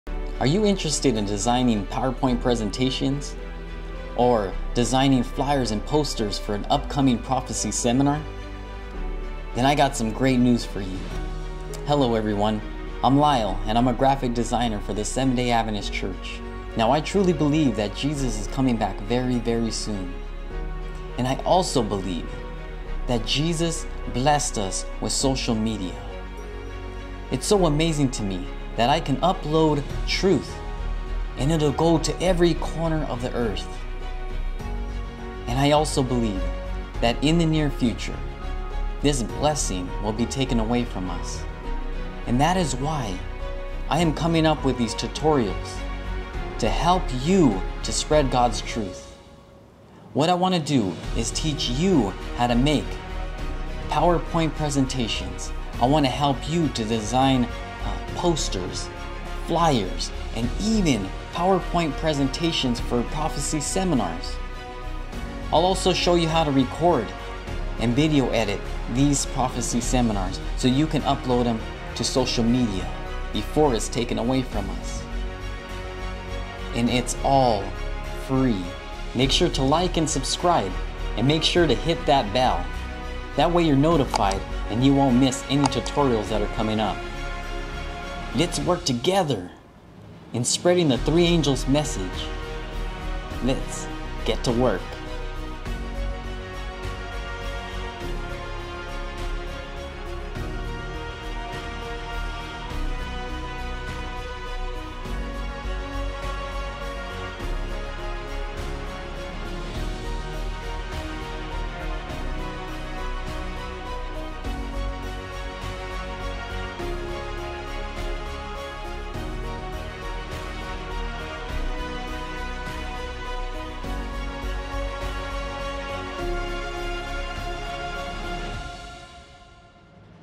Graphic Design Tutorials for Adventist
Free Graphic Design and Video Editing Tutorials.
Find out how to design flyers and poster for your pastor and church or for up coming Prophecy Seminars.
Find out how to design and use PowerPoint and more.